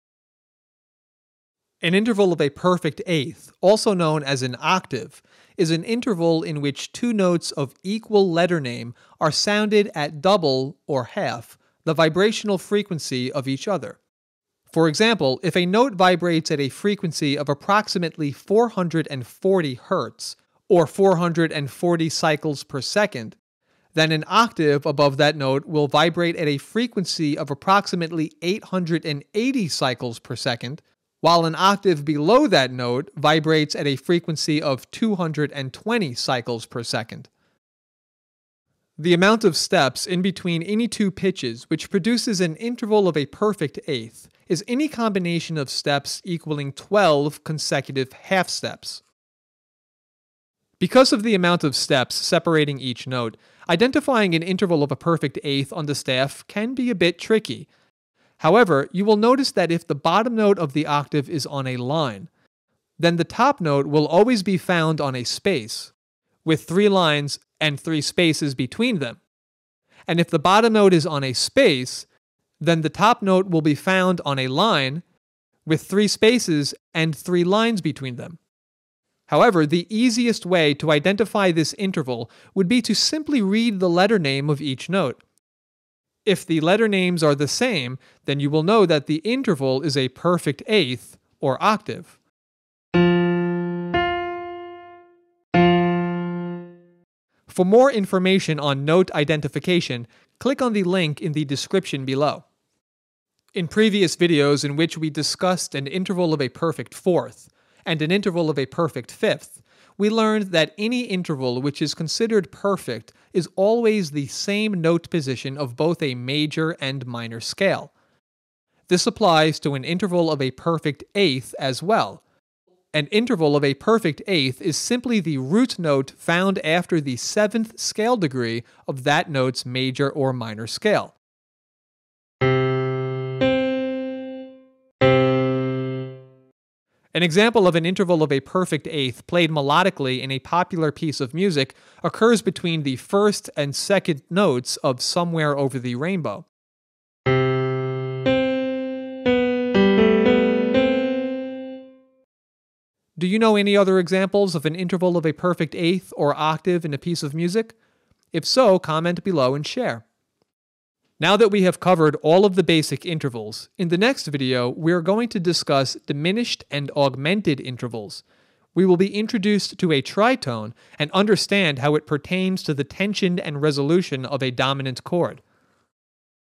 Intervals EXPLAINED!! | Perfect Eighths and Octaves
An interval is the distance from one note to the next note either above or below it.

In this video series we are going to discuss what constitutes each interval, see how they pertain to the scale, and learn how to properly read and write intervals on the staff. Additionally, we will go over a number of tips and tricks which will make working with intervals much easier.

Intervals made easy,
Music theory made easy,

Five Minute Mozart

Connect With Five Minute Mozart